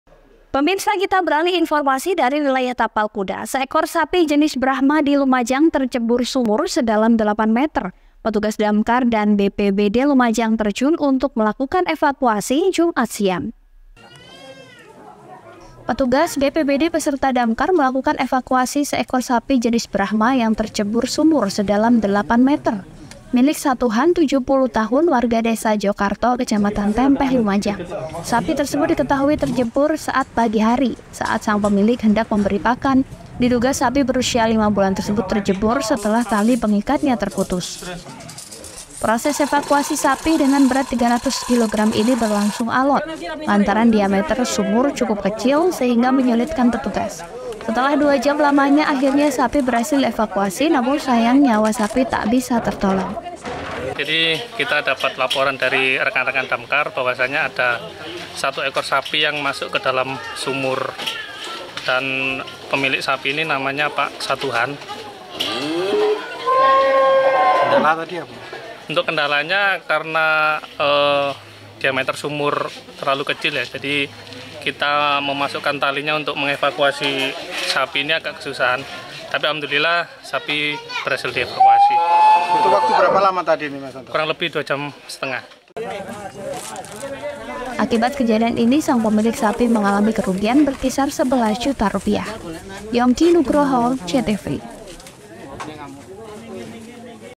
SAPI BRAHMA TERCEBUR SUMUR
PETUGAS BPBD BESERTA DAMKAR MELAKUKAN EVAKUASI SEEKOR SAPI JENIS BRAHMA YANG TERCEBUR SUMUR SEDALAM 8 METER, MILIK  SATUHAN 70 TAHUN WARGA DESA JOKARTO KECAMATAN TEMPEH LUMAJANG.

tiktok♪            : @jtvbanyuwangi
instagram📷 : @jtvbanyuwangi
facebook ⓕ   : JTVBANYUWANGI